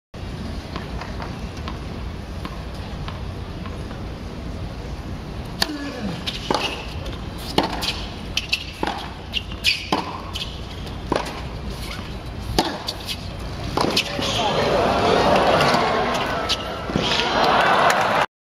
Marin Cilic - great service point into the net for finish
Citi Open 2015 semi finals. Rematch of US Open 2014 finals*